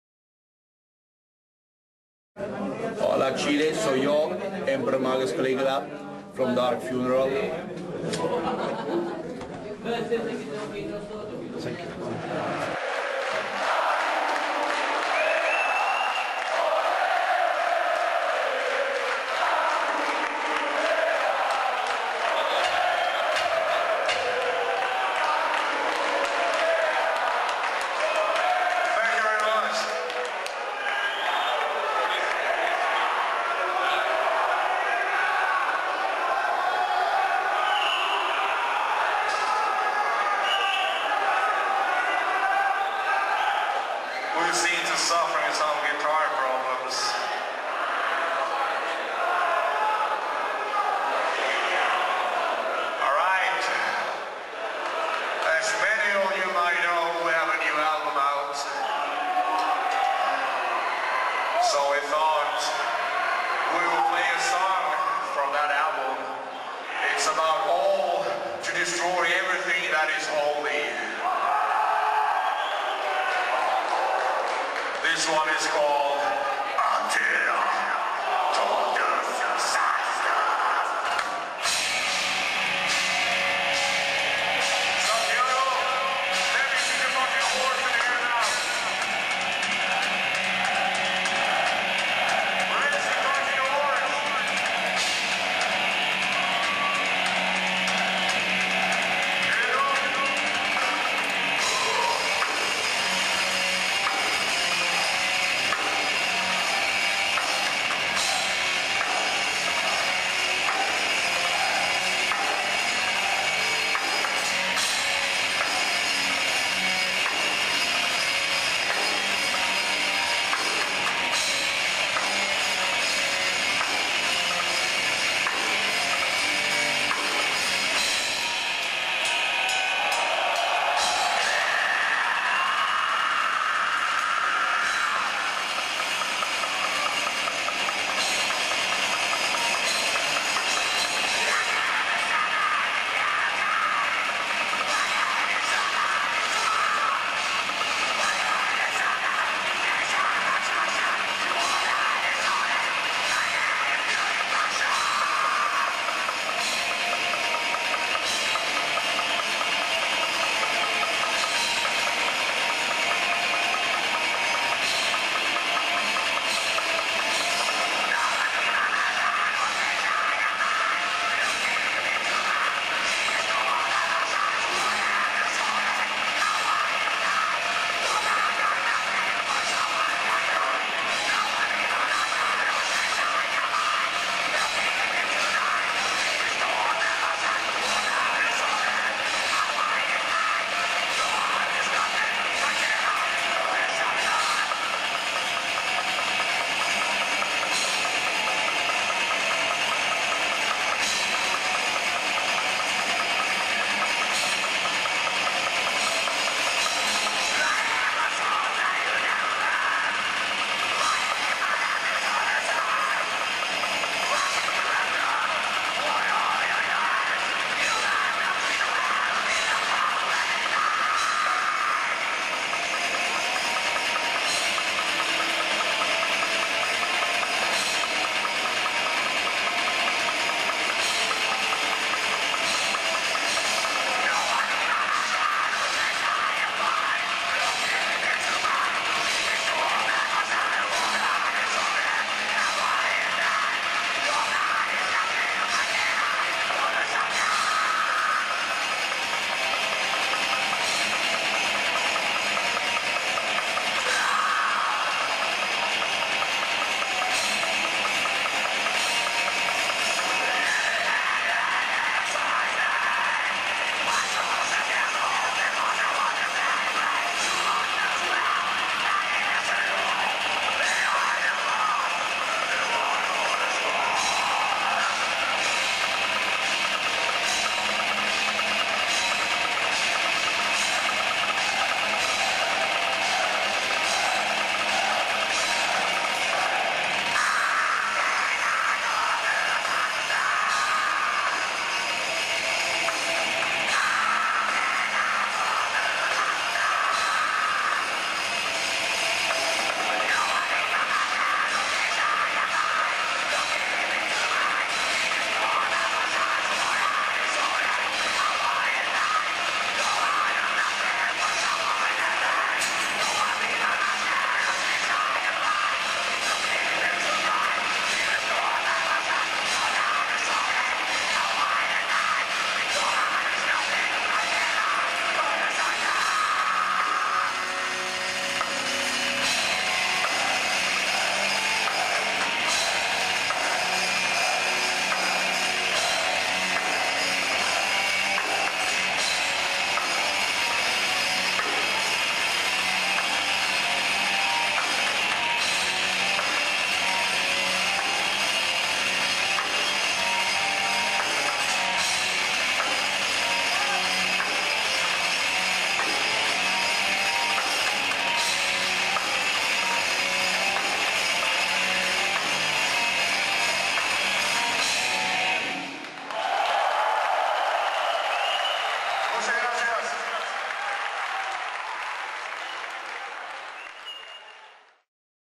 DARK FUNERAL EN CHILE 2006
Dark Funeral live en Teatro Novedades. Santiago Chile Octubre 7th, 2006